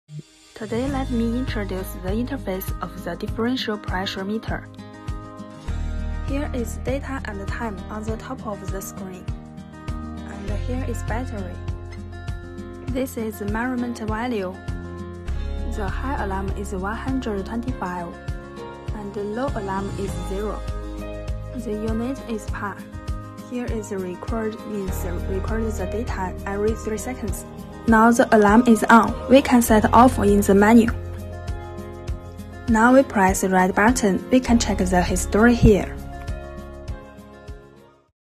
Zetron DP-40 Differential Pressure Meter Interface Introduction
In order to facilitate testing the air pressure difference between adjacent clean areas of different levels and between clean rooms and outdoors, the DP-40 differential pressure meter displays the indoor and outdoor air pressure difference in digital form.
Beijing Zetron Technology Co., Ltd